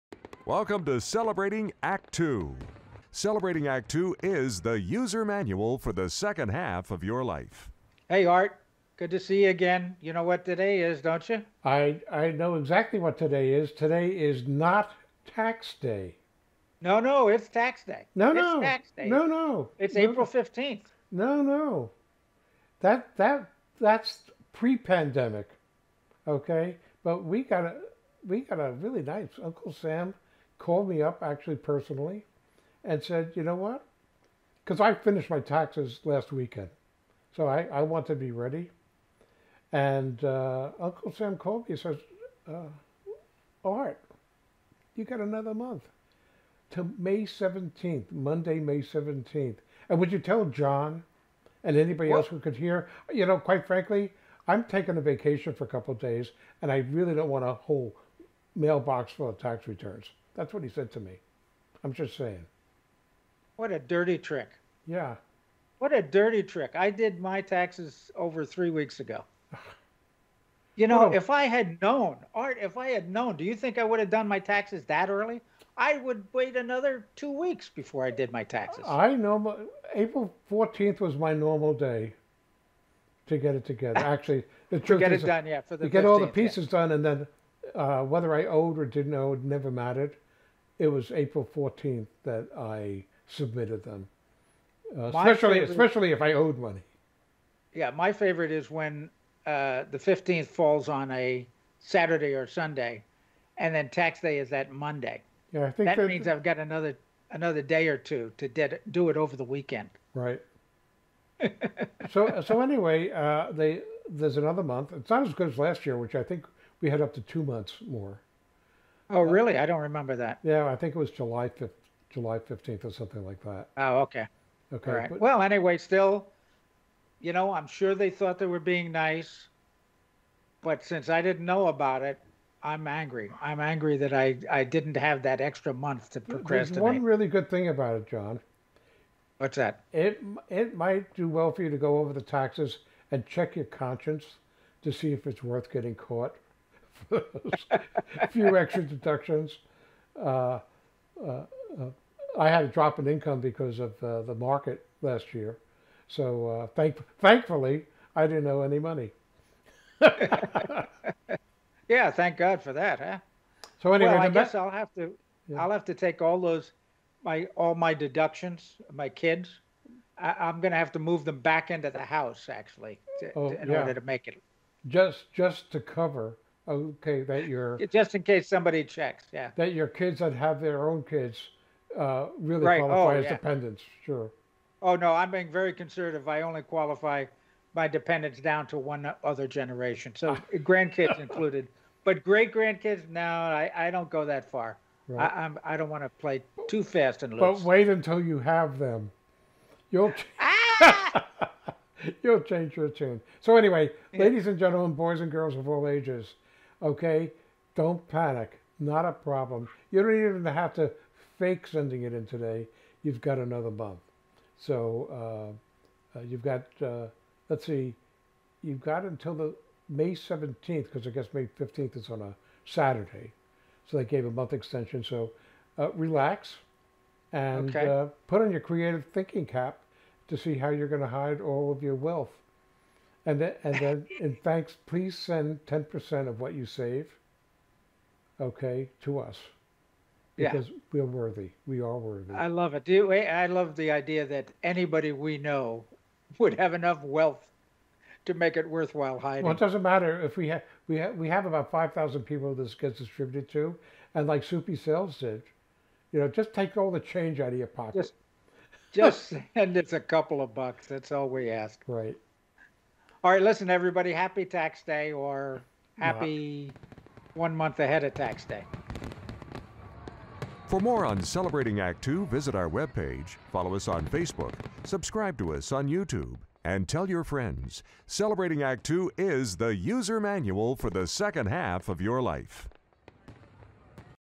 Tax Day 2021 - NOT!!!
Art & John got their Federal taxes ready to file on time on April 15 and wouldn't you know, Uncle Sam called and gave them until May 17, 2021.  Most states have adjusted their filing day as well.